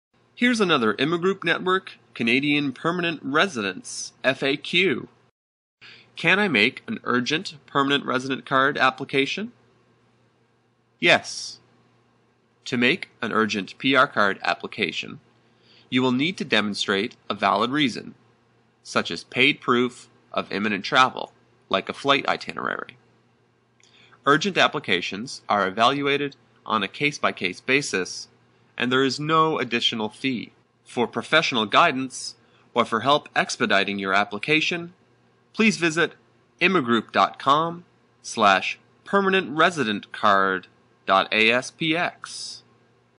Can I Make An Urgent Canadian Permanent Resident Card Application?
PLEASE NOTE: This video is out of date. Urgency processing has been restricted to very specific cases.

More questions? Call right now: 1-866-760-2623

Q: Can I make an urgent permanent resident card application?

A: Yes. To make an urgent PR card application you will need to demonstrate a valid
reason, such as paid proof of imminent travel like a flight itinerary. Urgent applications are evaluated on a case-by-case basis and there is no additional fee.
For professional guidance or for help expediting your application, please visit